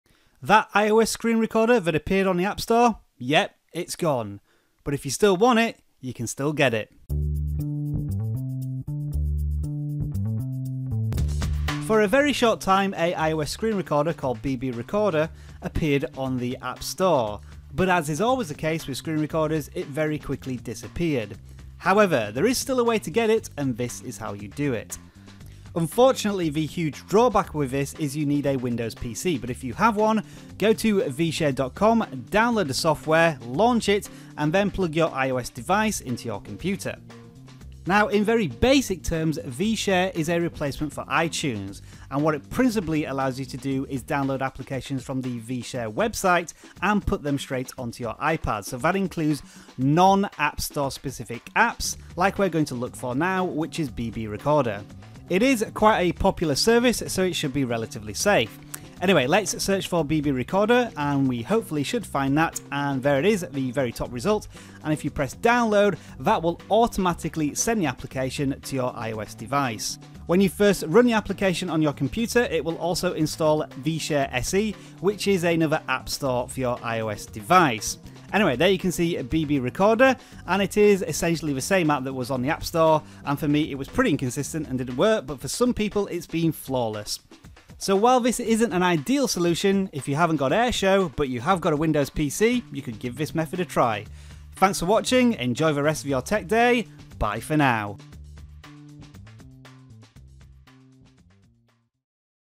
BB Recorder: Removed from the App Store but Still Alive! How to Record Your iPhone or iPad
With Airshou having troubles of its own, a free screen recorder burst onto the Apple app store called BB Recorder. And, as predicted, it quickly got removed.

So if you didn't have time to download it officially, you still have the opportunity to download it unofficially. Watch the video and see how.